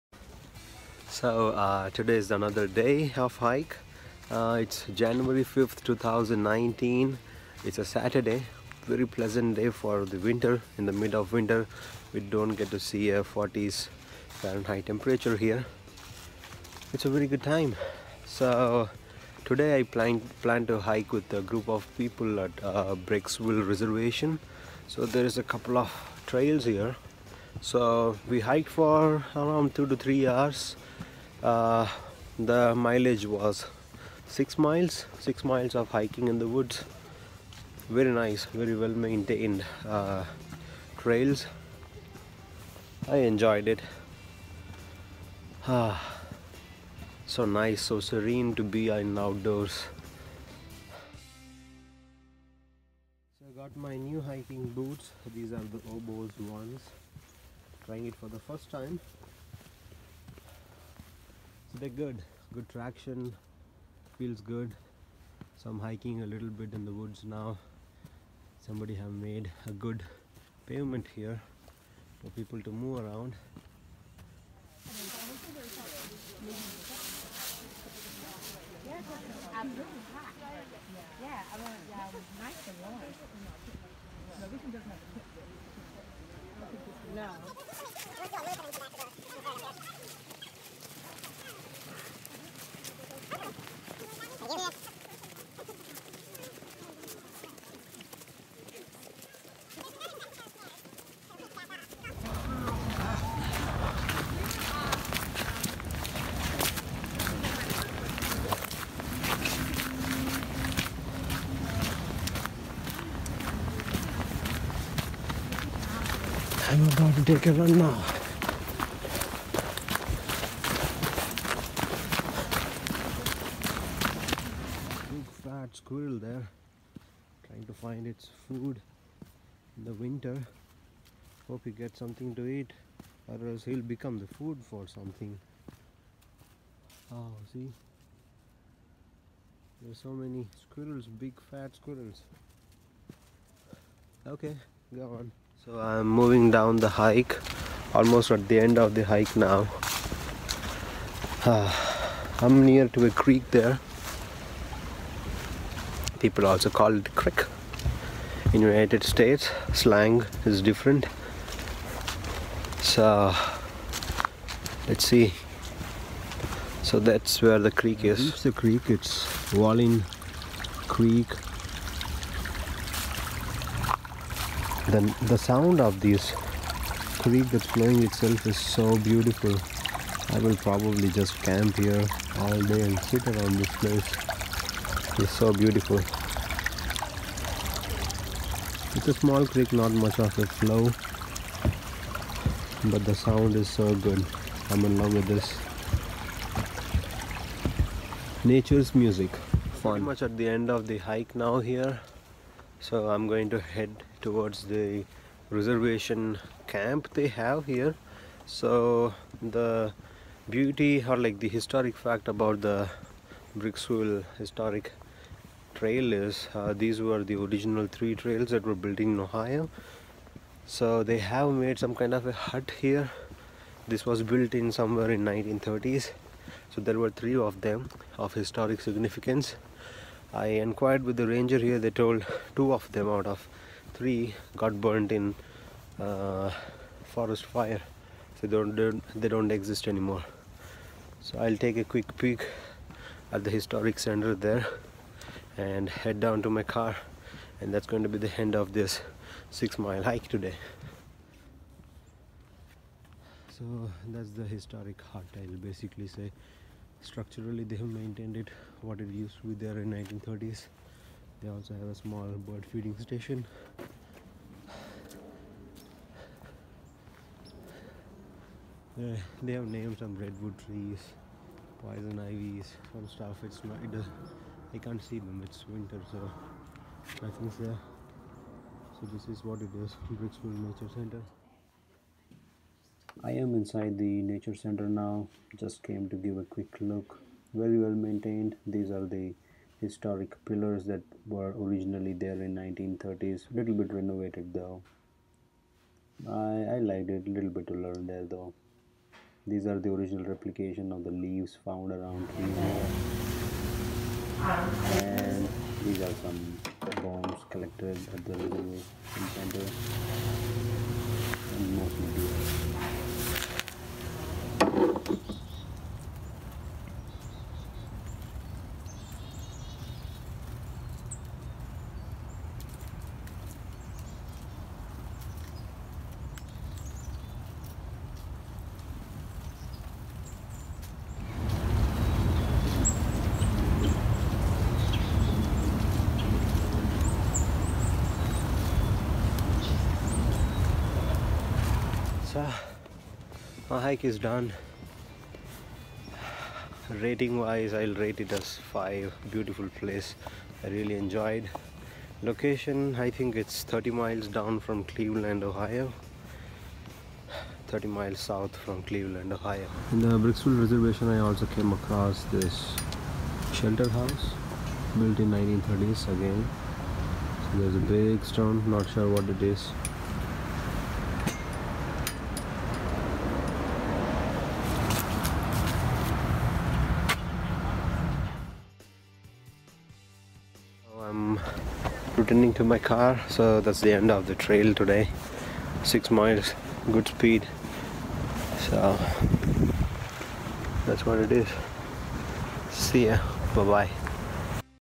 Brecksville Reservation - Buckeye Trail, Ohio, USA (6 Miles) Jan 5, 2019
My log at Brecksville Reservation - Buckeye Trail